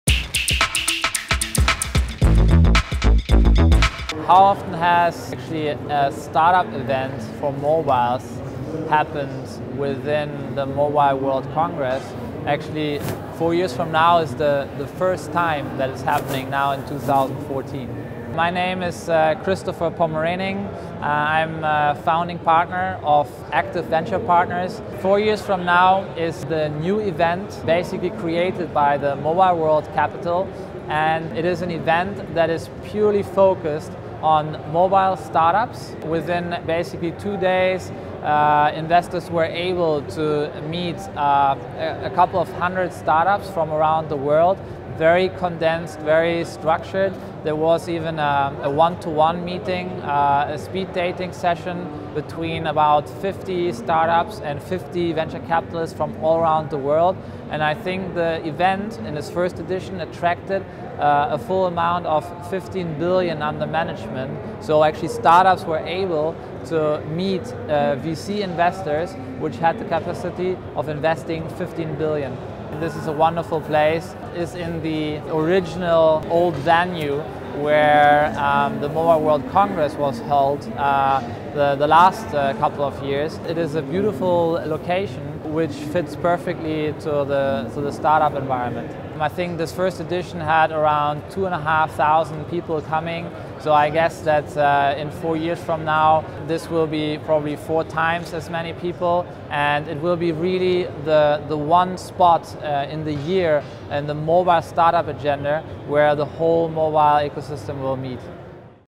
Startups Find a New Home at Mobile World Congress
Startups have taken over the old home of Mobile World Congress. Christopher Pommerening a venture capitalist and organizer of the Four Years From Now project explains why he wants get mobile startups and investors together in a single place.